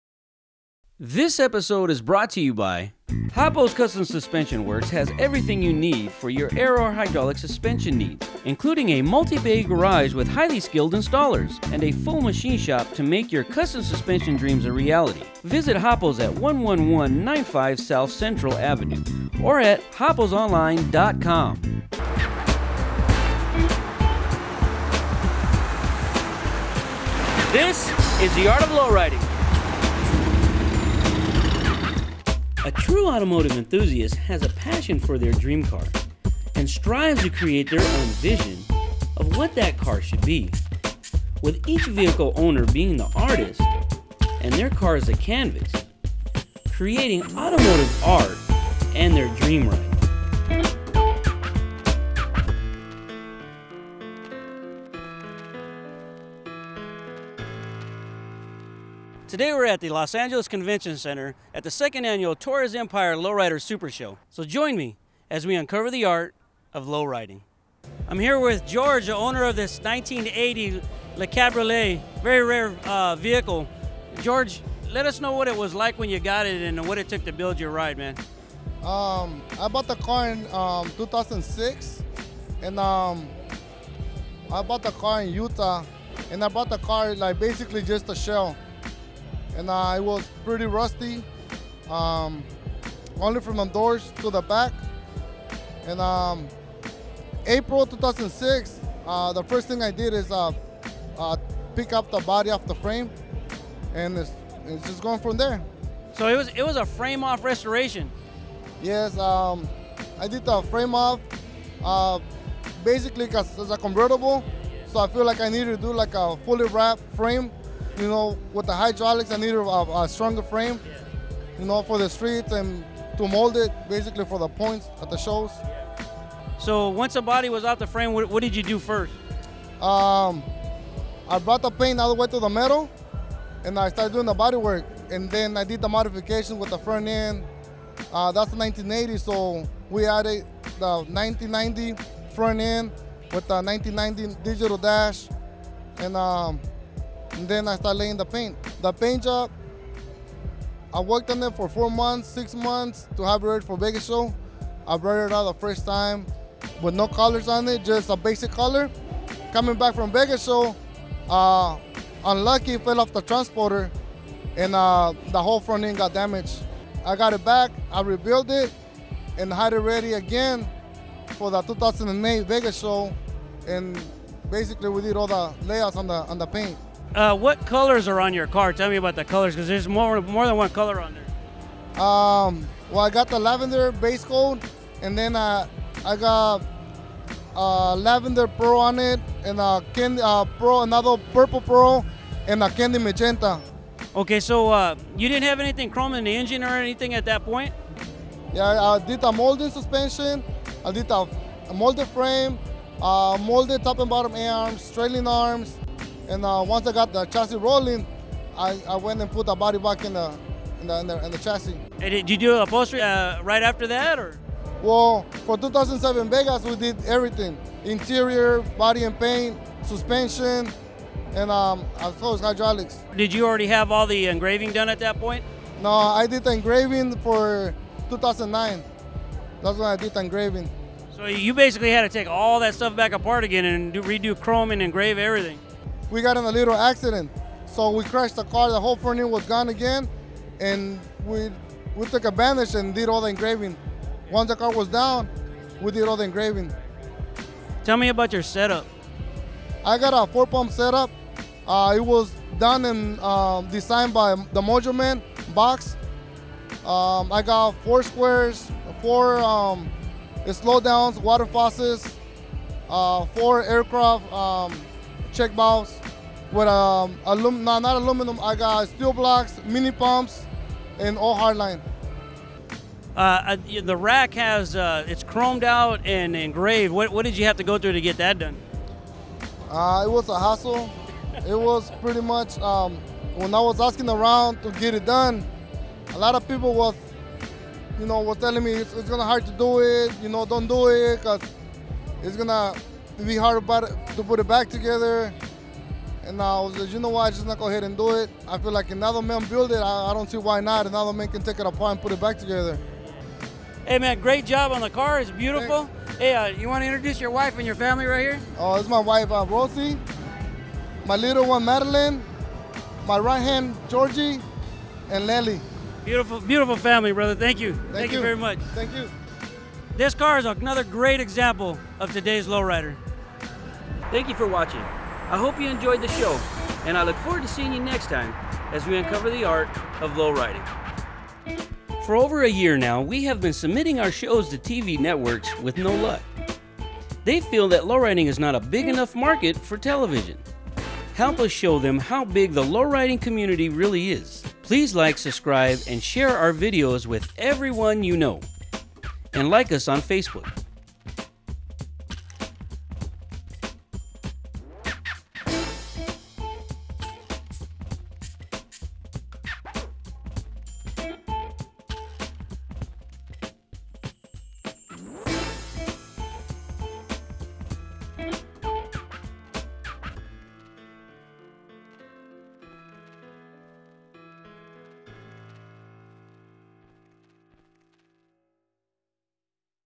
The Art of Lowriding 1980 Le Cabriolet Cadillac Fame
The Art of Lowriding interviewer with George Ramirez owner of this rare and beautiful 1980 Le Cabriolet Cadillac "Fame"